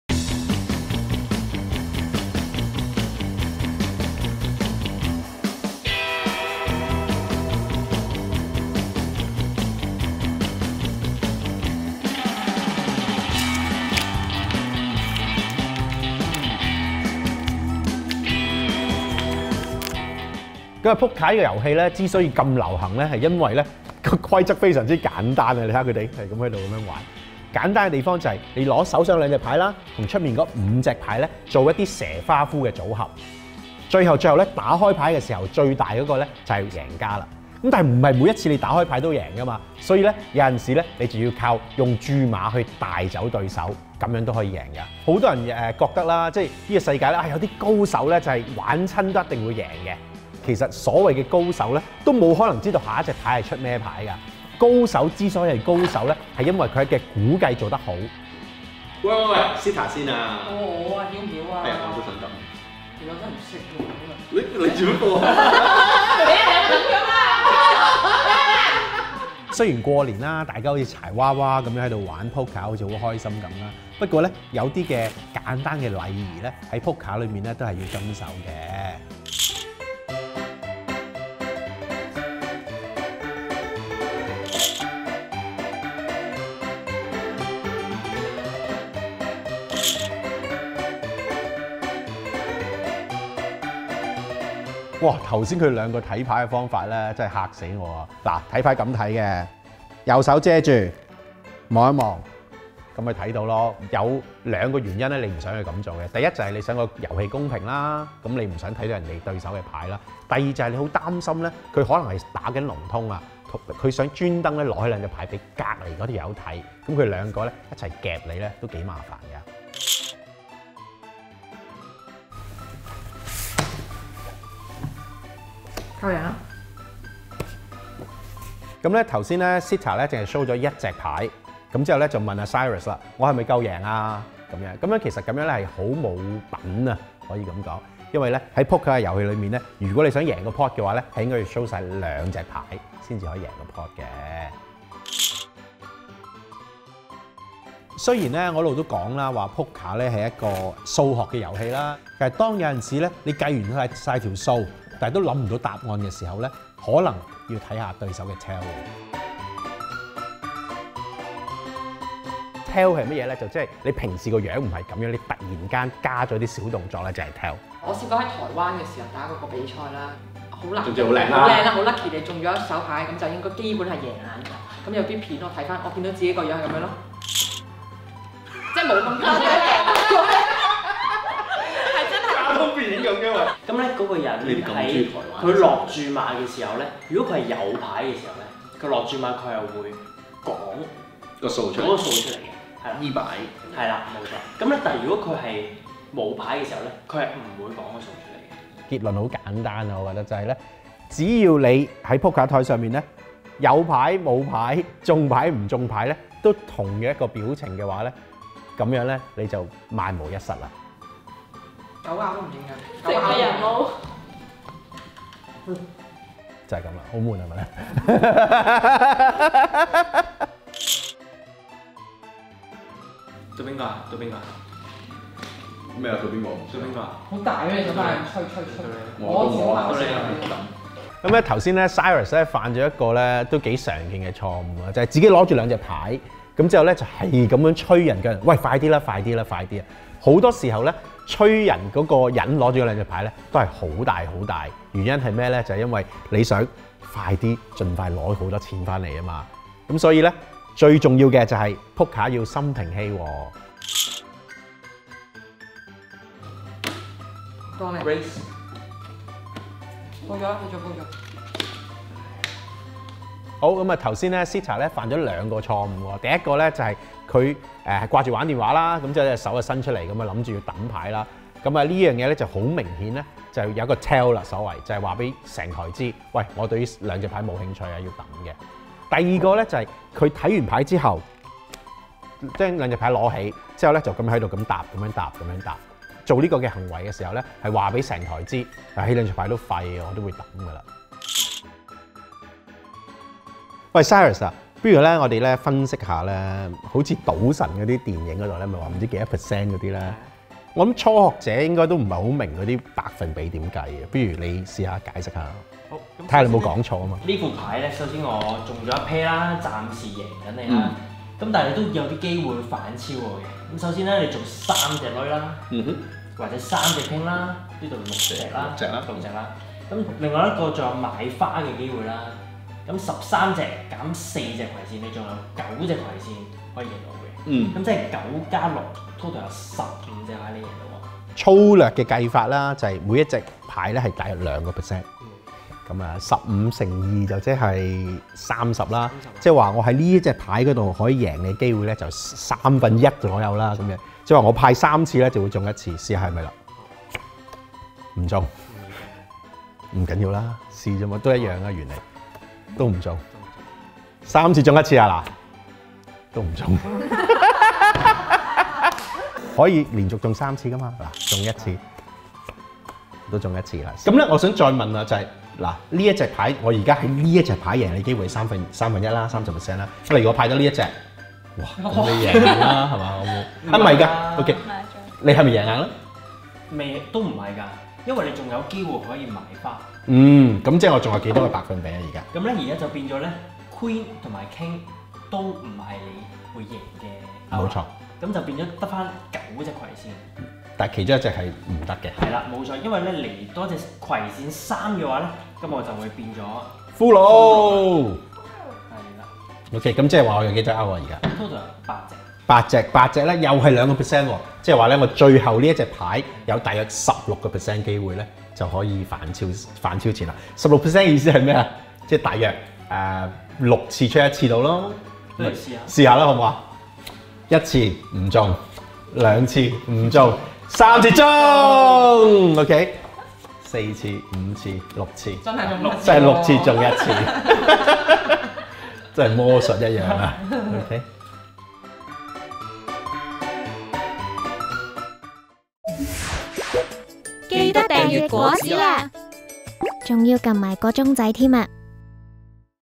賭撲克Poker要贏錢首先要識發吽哣！玩手機扮Chok開牌透露你是魚腩 捉心理下注碼計機率 新春賭局 專題─果籽 香港 Apple Daily─原刊日期：20210212
｜賭‧Poker｜玩手機 扮Chok開牌透露你是魚腩 捉心理下注碼計機率 要贏錢首先要識發吽哣
"過年限聚，大家都是留家看電視玩啤牌或玩Board Game。其中德州撲克（Poker/ 話事啤）就簡單又刺激。遊戲非常簡單，你拿手上的兩隻牌，與桌上的五隻牌作蛇花俘的組合，中間可以下注，最後開牌時誰最大是為勝出。但你不是一抽中好牌就稱王，或手執爛牌就棄牌（Fold）退堂，你可憑觀察，用注碼嚇走對方，一樣嬴。
黎上之( Stephen），是香港撲克牌手協會（HKPPA）董事。常跟會員們常去澳門台灣參加Poker比賽。在他們眼裏，這不是賭博遊戲，而是一場競技，「一個Poker玩得好的人，多數是數學不錯，心理質素好，最重要是識得自律的人。」新年流流大家都係圖開心，但若能學定幾招速成心理戰，牌局上勝算高啲造型又型啲。佢話好，但係「人品好牌品自然好，不如學啲牌品先。」"